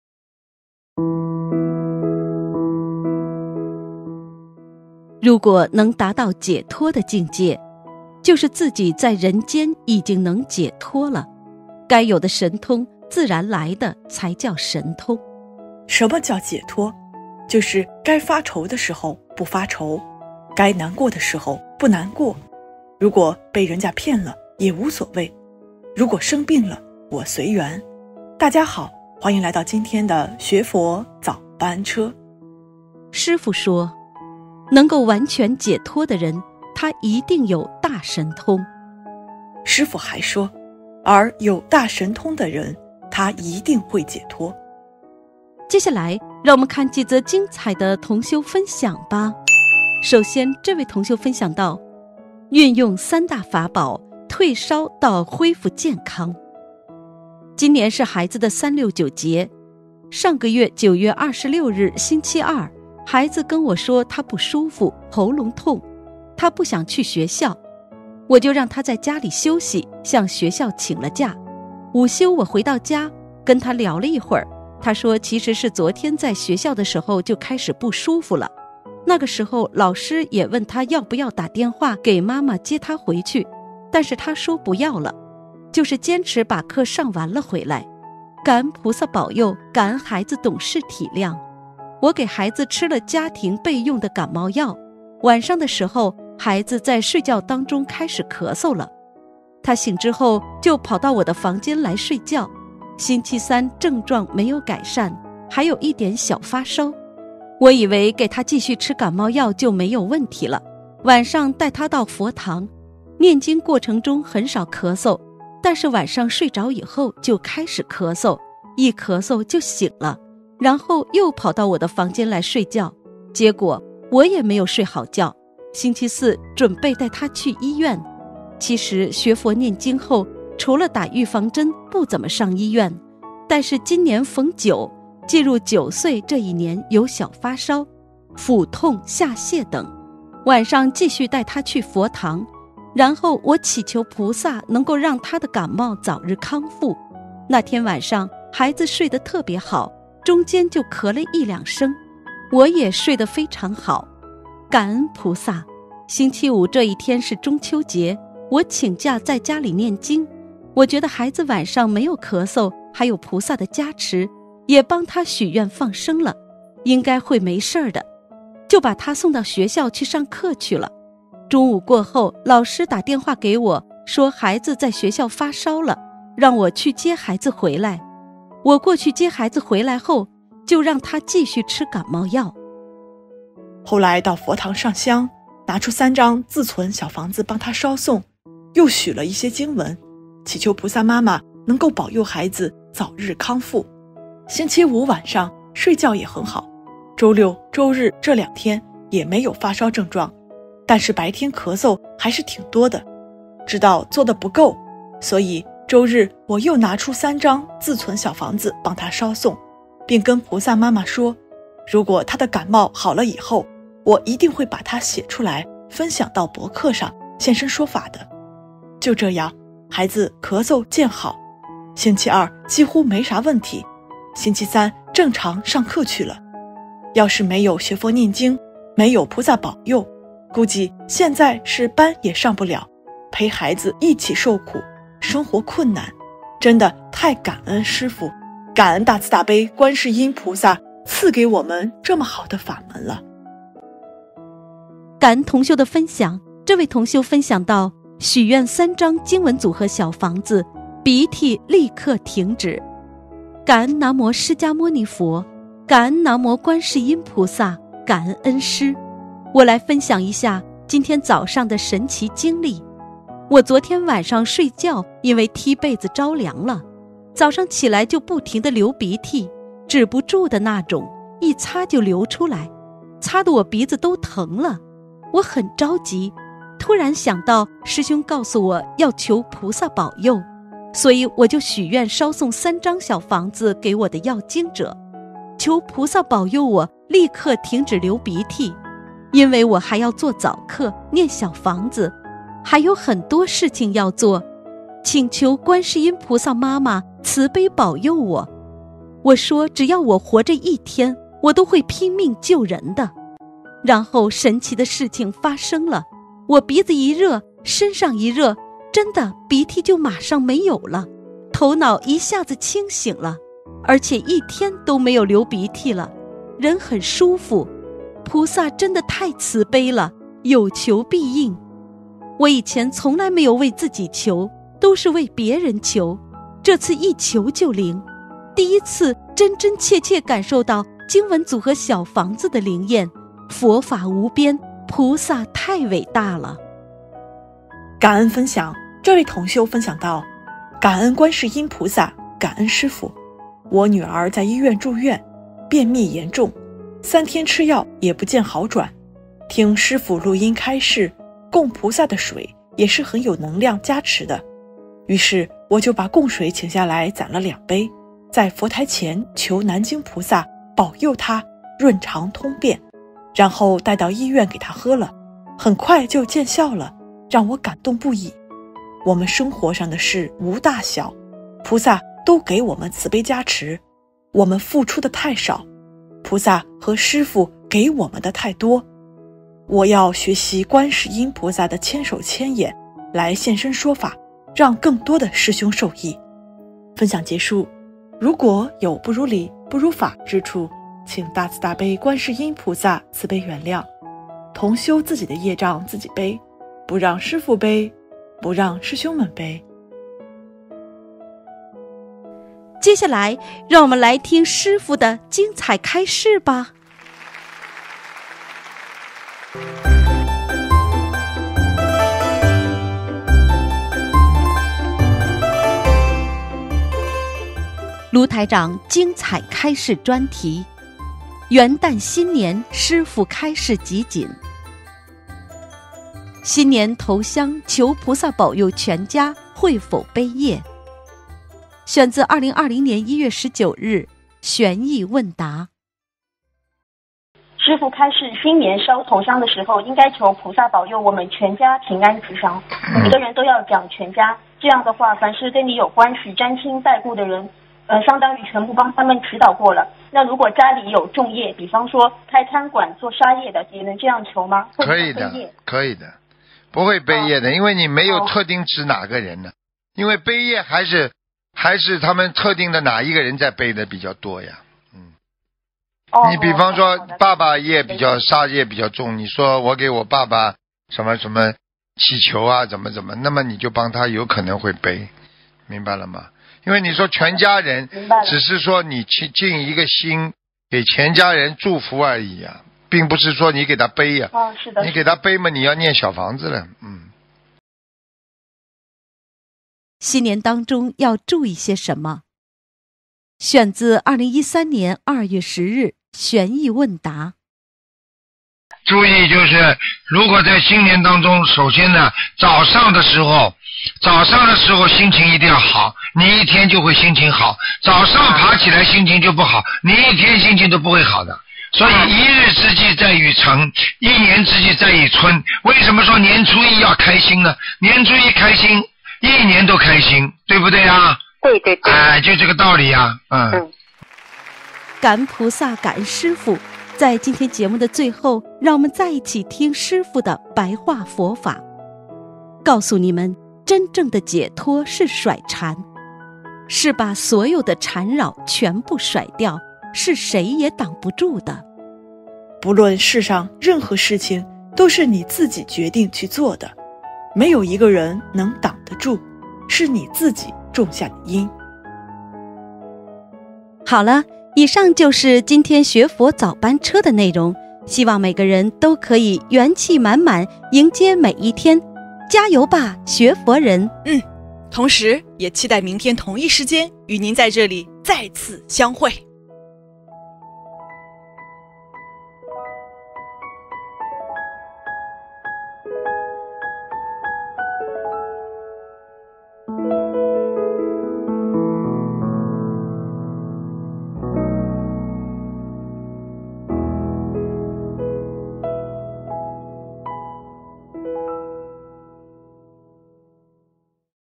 学佛早班车视频 第974期
🌈摘自师父《白话佛法》

如果能达到解脱的境界，就是自己在人间已经能解脱了，该有的神通，自然来的才叫神通。什么叫解脱？就是该发愁的时候，不发愁，该难过的时候，不难过，如果被人家骗了，也无所谓，如果生病了我随缘。能够完全解脱的人，他一定有大神通，而有大神通的人，他一定会解脱。

---《白话佛法》 第一册  35、谈缘份与神通

1.
运用“三大法宝”，退烧到恢复健康

今年是孩子的“三六九”劫。上个月9月26日（星期二），孩子跟我说他不舒服，喉咙痛，他不想去学校，我就在让他在家里休息，向学校请了假。午休我回到家，跟他聊了一会儿，他说其实是昨天在学校的时候就开始不舒服了，那个时候老师也问他要不要打电话给妈妈接他回去，但是他说不要了，就是坚持把课上完了回来。感恩菩萨保佑，感恩孩子懂事体谅。

我给孩子吃了家庭备用的感冒药。晚上的时候，孩子在睡觉当中开始咳嗽了。他醒之后就跑到我的房间来睡觉。星期三症状没有改善，还有一点小发烧。我以为给他继续吃感冒药就没有问题了。晚上带他到佛堂，念经过程中很少咳嗽。但是晚上睡着以后就开始咳嗽，一咳嗽就醒了，然后又跑到我的房间来睡觉，结果，我也没有睡好觉。星期四，准备带他去医院。

其实学佛念经后，除了打预防针不怎么上医院。但是今年逢九，进入九岁这一年有小发烧，腹子疼、下泻等。

晚上继续带他去佛堂，然后我祈求菩萨能够让他的感冒早日康复。那天晚上，孩子睡得特别好，中间就咳了一两声。我也睡得非常好，感恩菩萨。星期五这一天是中秋节，我请假在家里念经。我觉得孩子晚上没有咳嗽，还有菩萨的加持，也帮他许愿放生了，应该会没事的，就把他送到学校去上课去了。中午过后，老师打电话给我，说孩子在学校发烧了，让我去接孩子回来。我过去接孩子回来后，就让他继续吃感冒药。

2.
后来到佛堂上香，拿出3张自存小房子帮他烧送，又许了一些经文，祈求菩萨妈妈能够保佑孩子早日康复。星期五晚上睡觉也很好。周六周日这两天也没有发烧症状，但是白天咳嗽还是挺多的，知道做得不够，所以周日我又拿出3张自存小房子帮他烧送，并跟菩萨妈妈说，如果他的感冒好了之后，我一定会把它写出来分享到博客上，现身说法的。就这样，孩子咳嗽渐好，星期二几乎没啥问题，星期三正常上课去了。要是没有学佛念经，没有菩萨保佑，估计现在是班也上不了，陪孩子一起受苦，生活困难。真的太感恩师父，感恩大慈大悲观世音菩萨赐给我们这么好的法门了。

3.
许愿3张经文组合小房子，鼻涕立刻停止

感恩南无释迦牟尼佛！感恩南无观世音菩萨！感恩恩师！

我来分享一下今天早上的神奇经历。我昨天晚上睡觉因为踢被子着凉了，早上起来就不停地流鼻涕，止不住的那种，一擦就流出来，擦得我鼻子都疼了。

我很着急，突然想到师兄告诉我要求菩萨保佑，所以我就许愿烧送3张小房子给我的要经者，求菩萨保佑我立刻停止流鼻涕，因为我还要做早课，念小房子，还有很多事情要做，请求观世音菩萨妈妈慈悲保佑我。我说只要我活着一天，我都会拼命救人的。

然后神奇的事情发生了，我鼻子一热，身上一热，真的鼻涕就马上没有了，头脑一下子清醒了，而且一天都没有流鼻涕了，人很舒服！菩萨真的太慈悲了，有求必应。

我以前从来没有为自己求，都是为别人求。这次一求就灵，第一次真真切切感受到经文组合小房子的灵验，佛法无边，菩萨太伟大了！

4.
感恩观世音菩萨，感恩师父。

我女儿在医院住院，便秘严重，三天吃药也不见好转。听师父录音开示，供菩萨的水也是很有能量加持的。于是我就把供水请下来攒了两杯，在佛台前求南京菩萨保佑她润肠通便，然后带到医院给她喝了，很快就见效了，让我感动不已。我们生活上的事无大小，菩萨都给我们慈悲加持，我们付出的太少，菩萨和师父给我们的太多。我也要学习观世音菩萨的千手千眼来现身说法，让更多的师兄受益。

🌈点亮心灯 开悟人生——师 父 精 彩 开 示

元旦新年师父开示集锦

新年头香求菩萨保佑全家会否背业
听众：师父开示：新年烧头香的时候，应该求菩萨“保佑我们全家平安吉祥”。每个人都要讲“全家”，这样的话，凡是跟你有关系、沾亲带故的人，相当于全部帮他们祈祷过了。那如果家里有重业，比方说开餐馆、做杀业的，也能这样求吗？会不会背业？
台长：可以，不会背业的，因为你没有特定指哪个人。“背业”是他们特定的哪一个人，才背得比较多。比方说爸爸杀业比较重，你说“我给我爸爸祈求”，那你就有可能会帮他背；你说“全家人”，只是说你去尽一个心给全家人祝福而已，并不是说你给他背，你给他背嘛你要念小房子了。
——2020年1月19日《玄艺问答》

新年当中要注意些什么
台长：首先早上的时候心情一定要好，你一天就会心情好。早上爬起来心情不好，一天心情都不会好的。所以一日之季在于晨，一年之季在于春。为什么说年初一要开心呢，年初一开心，一年都开心，就这个道理啊！
——2013年2月10日《玄艺问答》

🌈结尾摘自师父《白话佛法》

告诉你们：真正的解脱是甩缠，是把所有的缠扰全部甩掉，是谁也挡不住的，不论世上任何事情都是你自己决定去做的，没有一个人能挡得住，是你自己种下的因。